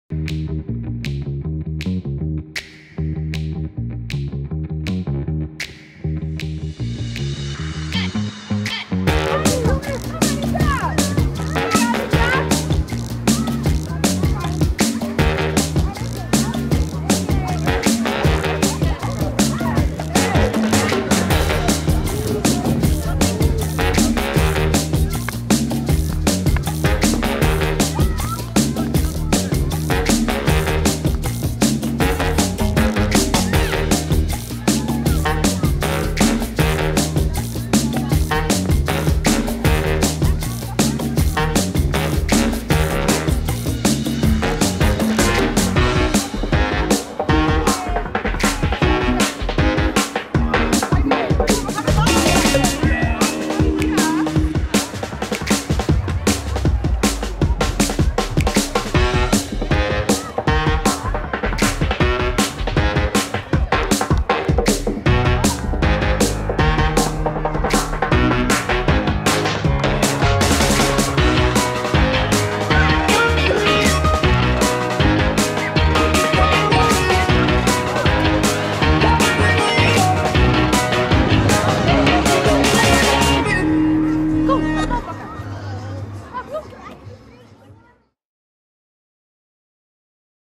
360 graden film Opening Hockey seizoen 2019-2020 Kromhouters
Op 31 augustus en 1 september vond de opening van het nieuwe derde veld en het funky veldje plaats bij de Kromhouters in Tiel. Deze snel geschoten 360 graden filmpjes geven een grappig inzicht binnen de clinics. Zoek je kind :-)